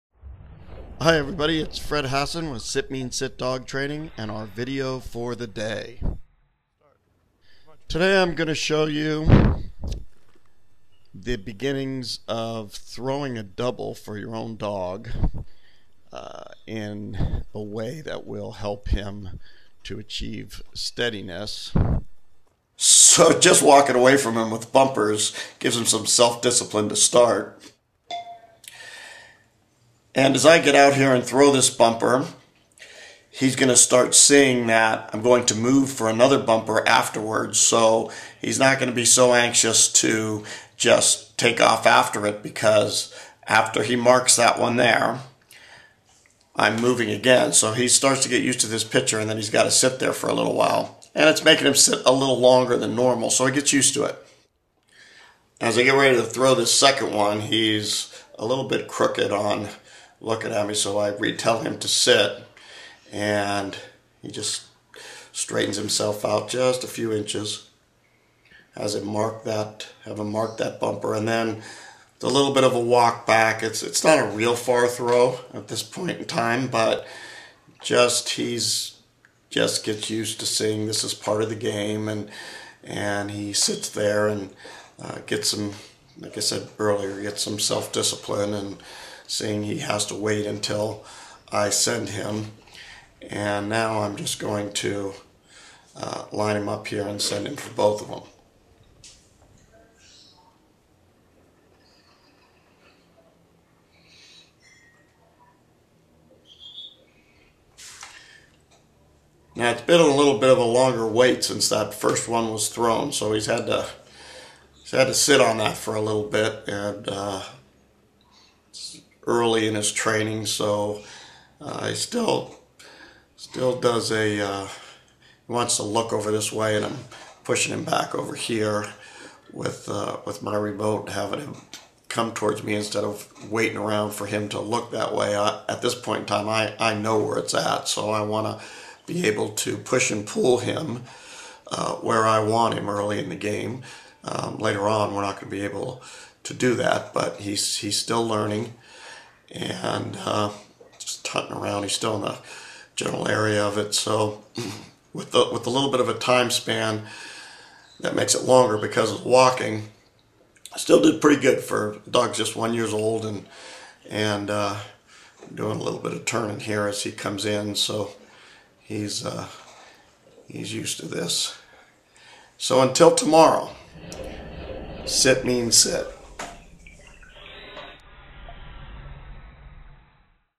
Retriever Training - Throwing doubles for your dog
In this video I show how I throw doubles in the early stages with my young black Labrador Retriever who is 1 year old.  This early drill done this way helps to build steadiness.  There are lots of variations you can use also.